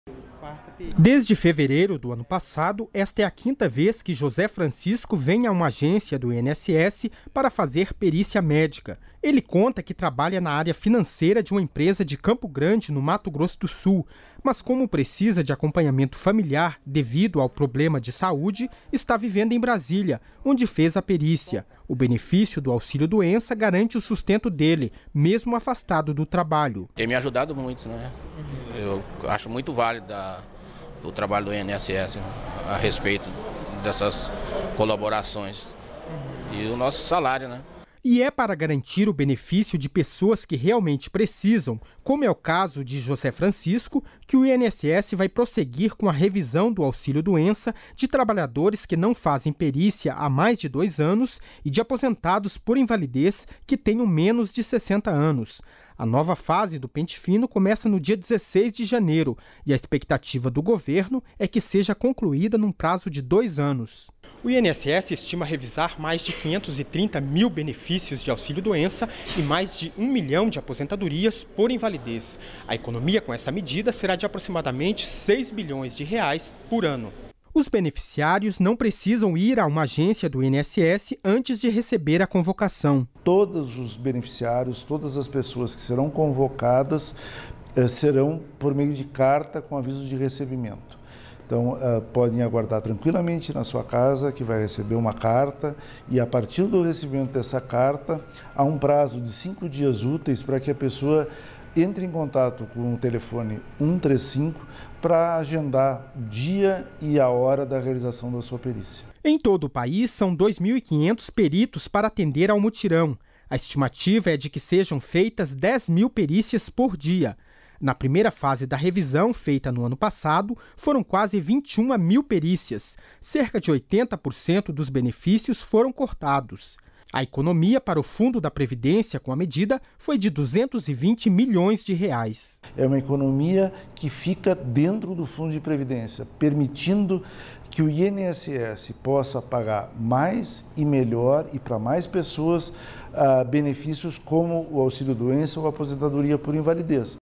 INSS faz pente-fino contra fraude no pagamento de auxílio-doença
REPÓRTER NBR - 11.01.17: O pente fino em cima dos benefícios concedidos pelo INSS vai ser retomado no dia 16 de janeiro. Cerca de 530 mil pessoas que recebem auxílio-doença e estão a mais de dois anos sem perícia, serão chamadas para uma avaliação. O objetivo é redirecionar os recursos previdenciários para quem realmente precisa.